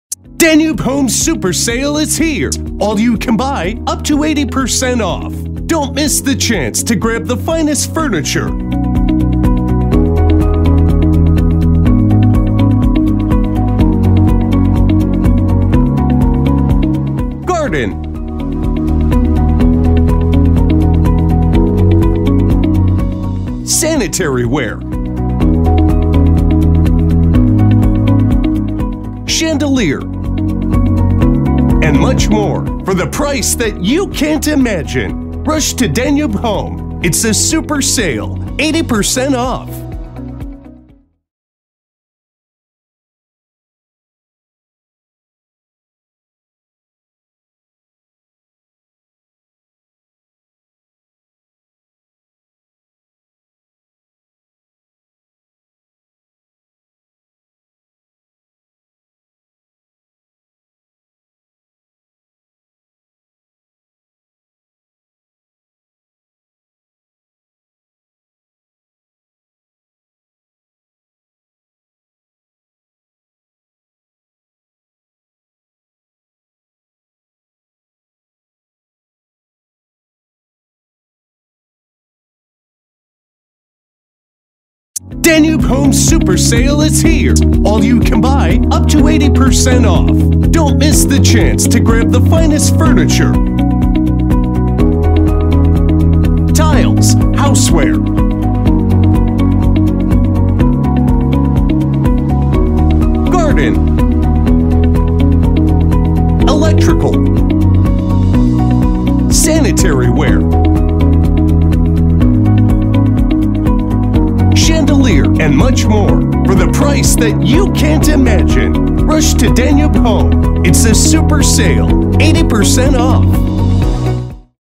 Danube Home Super Week in Oman
أسبوع التخفيضات الكبرى عاد مجدداً! توفر لك دانوب هوم عروض كومبو ممتازة وخصومات أفضل مذهلة. أسبوع من العروض الرائعة حتى 80٪ خصم على منتجاتك المفضلة. تسوق الآن ! ابتداءً من 25 أبريل 2019
Super Week is back again!!
Danube Home brings to you excellent combo offers and even better discounts. One week of amazing deals up to 80%OFF on your favorite products. Starting from 25th April 2019